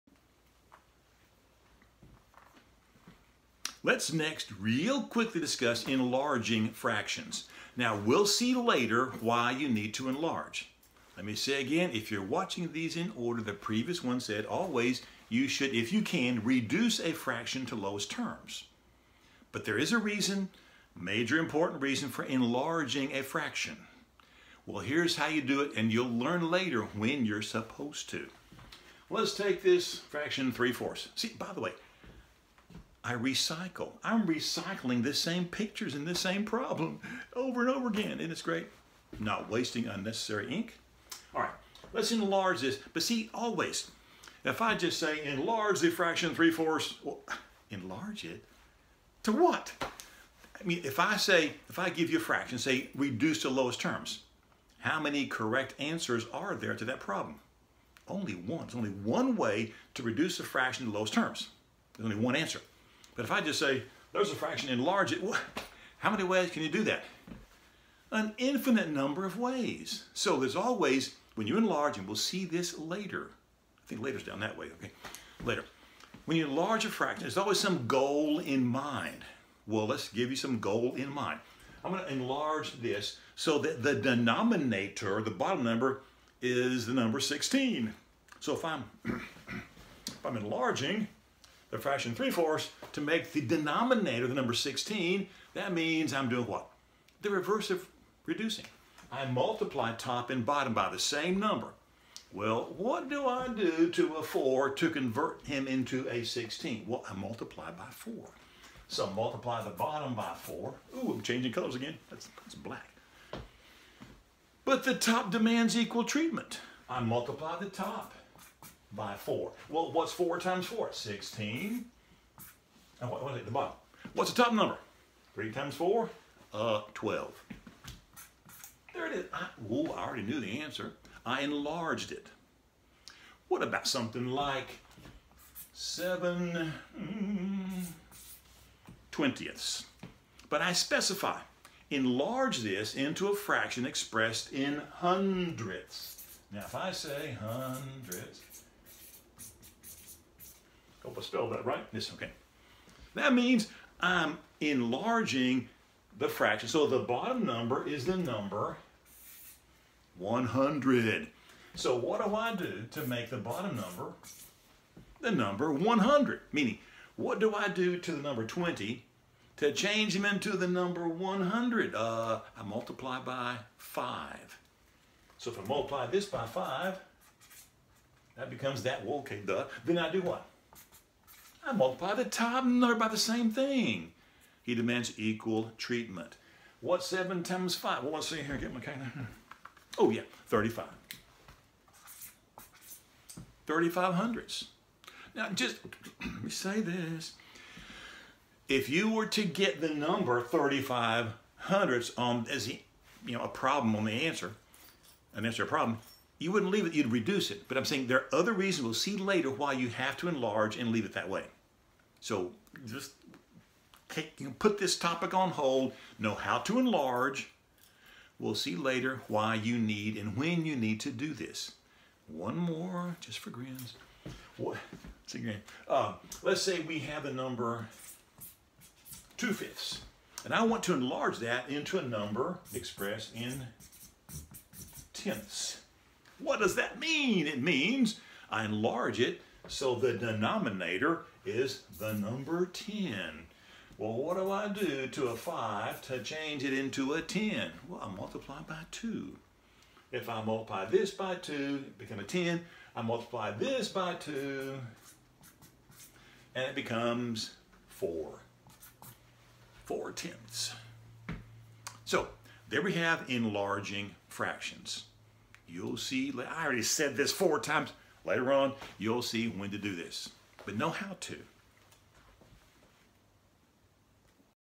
Enlarging Fractions
A tutorial about how to enlarge fractions.